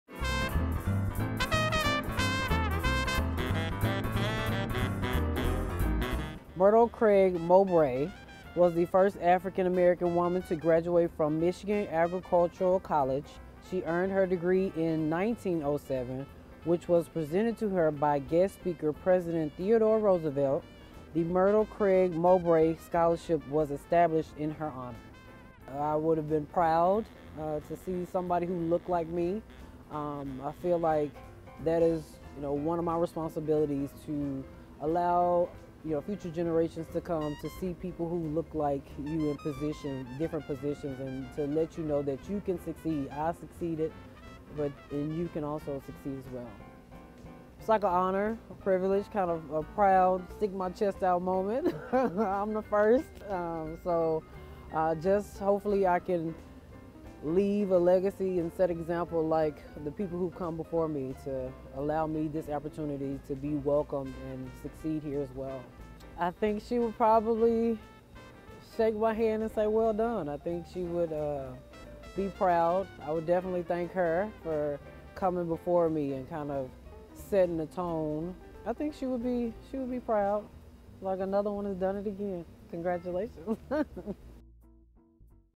Black History - Myrtle Craig Mowbray
Talesha Dokes talks about the first African-American female graduate from what was then Michigan Agricultural College, and how she helped blaze a trail for future students